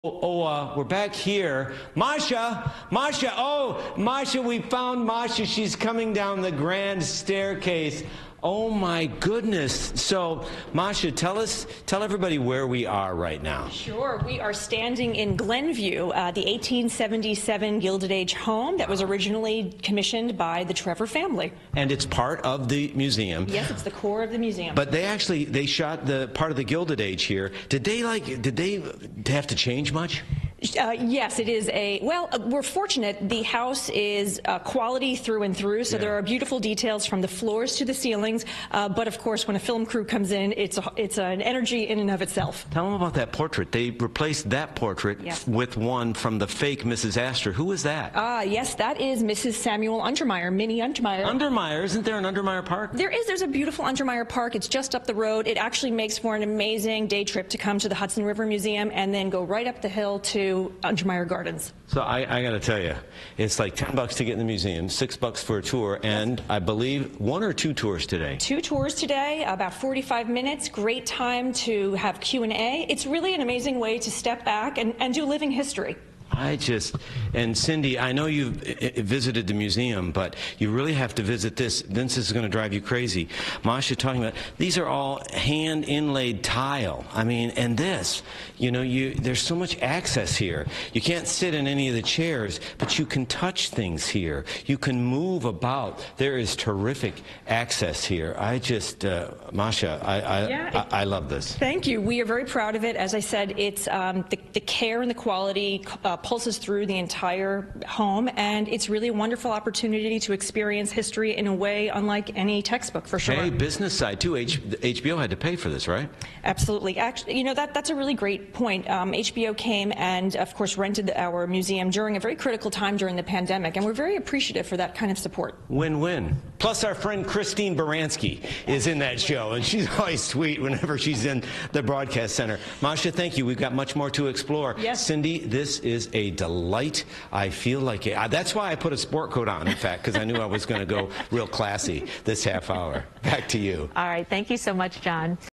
CBS New York at Hudson River Museum—Glenview Historic Home
On Sunday, March 6, 2022, John Elliott from CBS New York joined HRM Director & CEO Masha Turchinsky to explore HRMGlenview, our historic Gilded Age home, which was recently featured on the HBO hit series "The Gilded Age."

Glenview is an 1877 Gilded Age home on the National Register of Historic Places, designed by architect Charles W. Clinton. It was the home of John Bond Trevor, a highly successful stockbroker, who, like many other businessmen of the late nineteenth century, preferred to live in comfort in the newly accessible suburbs of New York City.

Guided tours of Glenview are available Thursday - Sunday.

Support the HRM today! Learn all about the benefits of membership